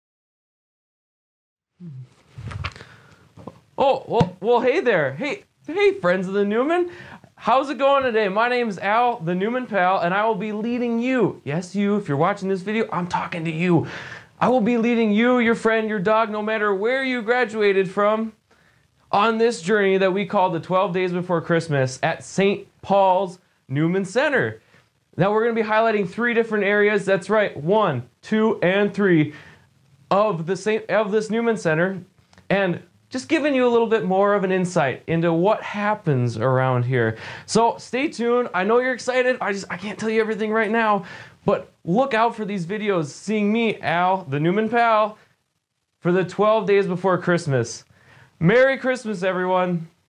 Introducing: Al the Newman Pal!
Al the Newman Pal is going to take us through the 12 days before Christmas by highlighting different aspects of our building and to raise awareness for our $2 million campaign match that goes through December 31st, 2017.